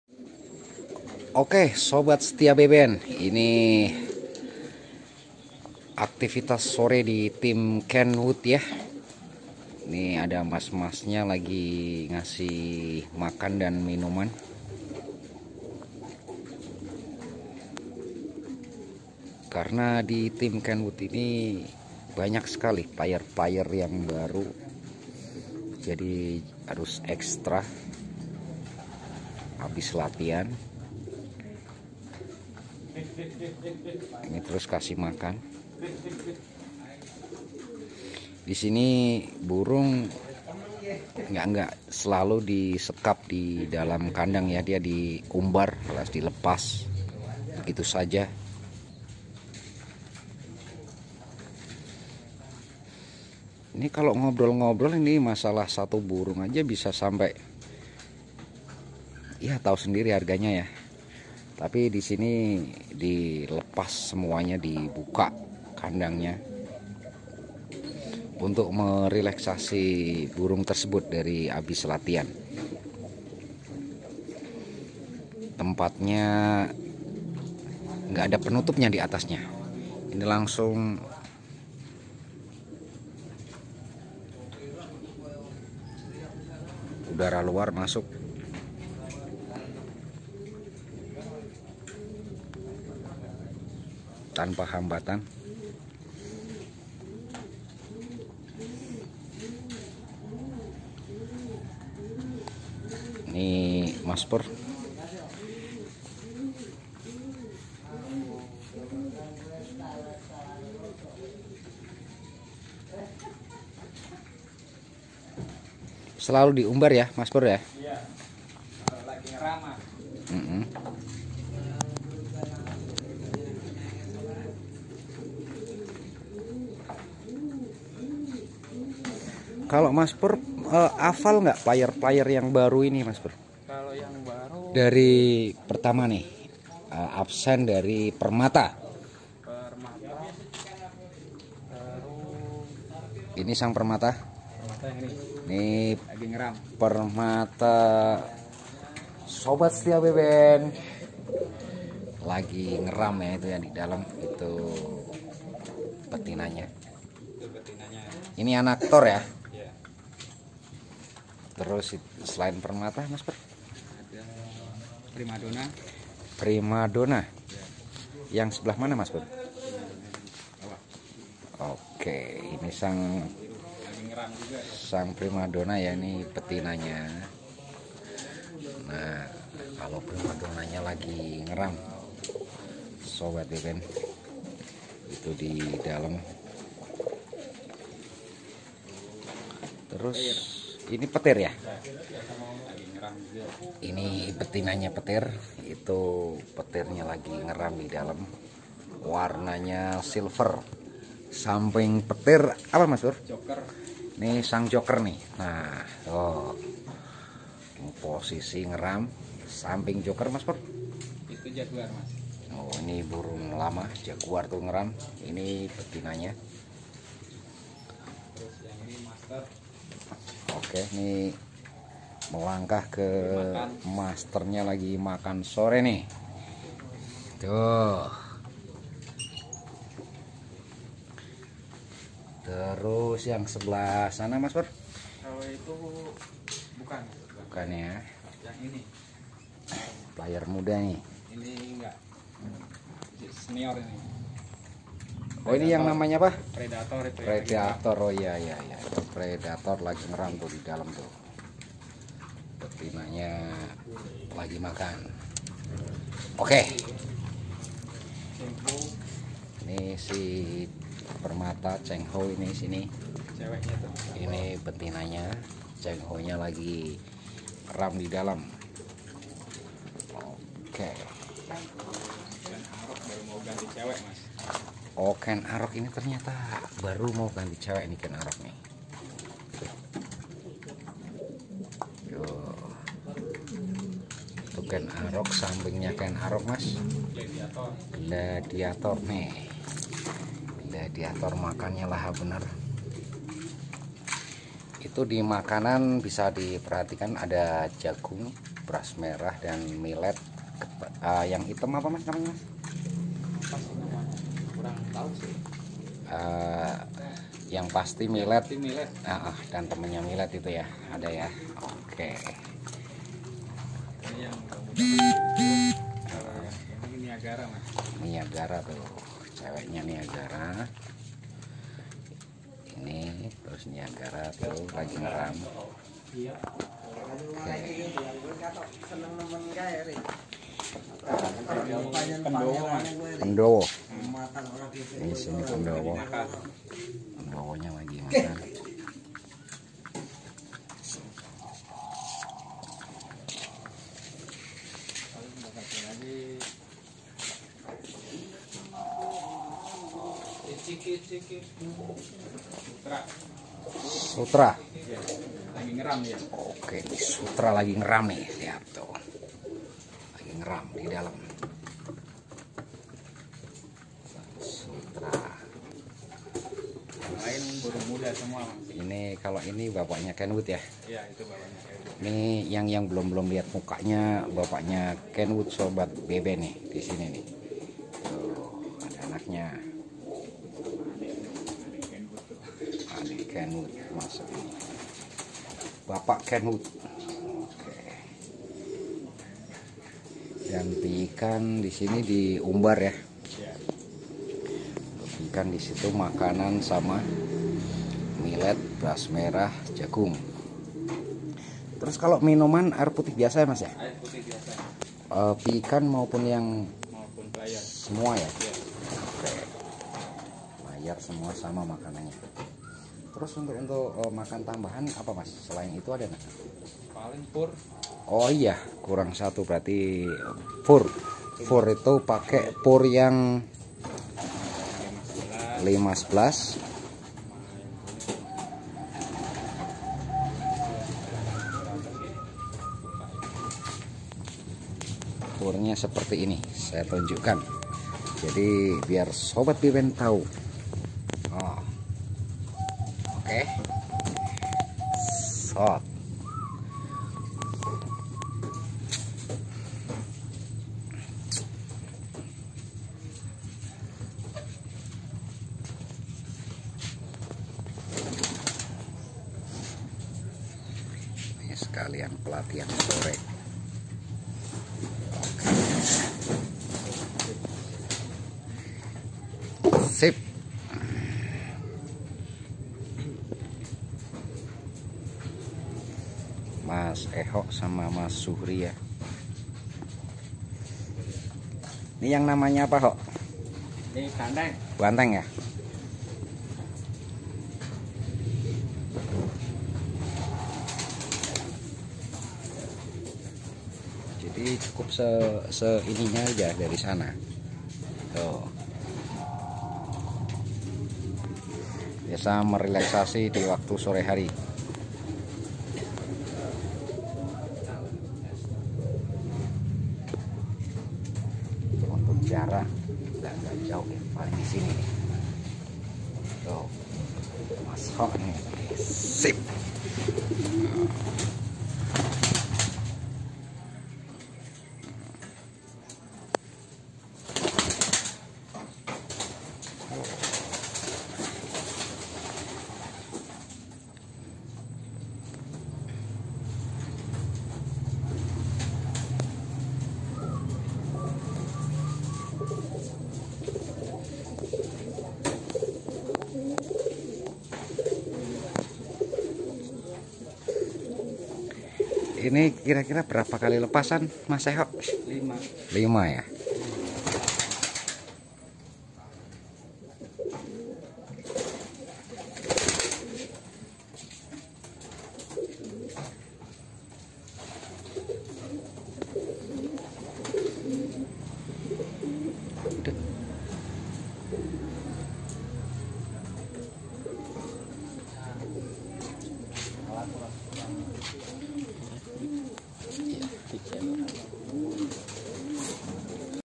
Player Player Terbaru Team Kenwood | Perawatan Sore Hari di kandang Team Kenwood
Player muda terbaru dari team kenwood